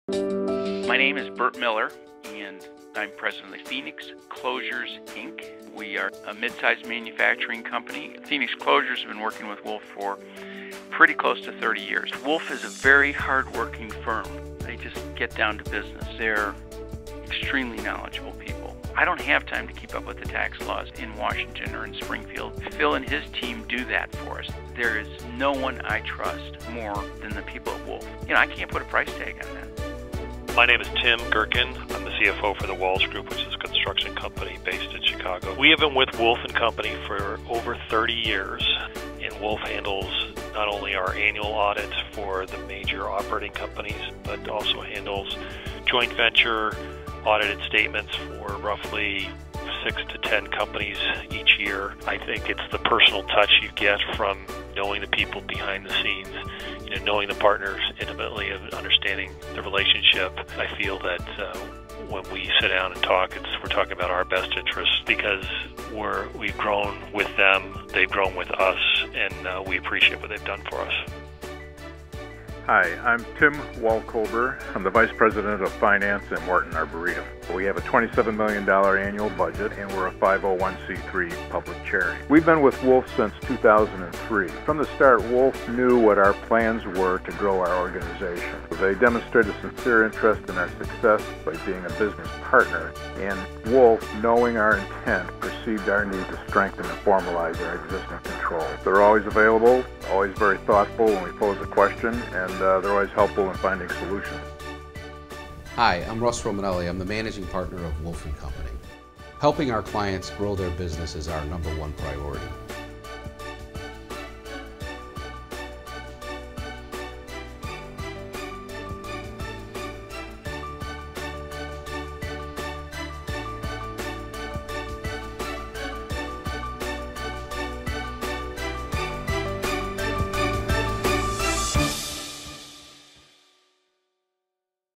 Wolf & Company - Accountants. Consultant. Practical Business Solutions - Expect More
Wolf & Company LLP is one of the largest CPA firms in metropolitan Chicago. Founded in 1978 as an independent certified public accounting firm, the firm is headquartered in Oakbrook Terrace, Illinois.

Today, Wolf's professionals and support personnel serve nearly 2,000 clients, providing a comprehensive array of accounting, tax and consulting services.